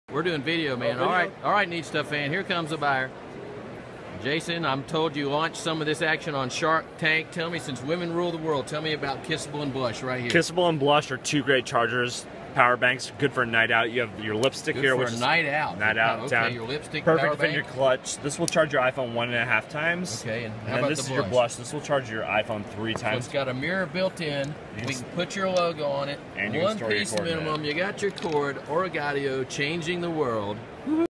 Lipstick or Phone Charger?
...either way, you look MARVELOUS! A compact or lipstick. smart device charger!?! Jason the Juice Box guy of trendsetting, butt-kicking ORIGAUDIO shares some Neat New Stuff for 2015! Making your brand spectacular @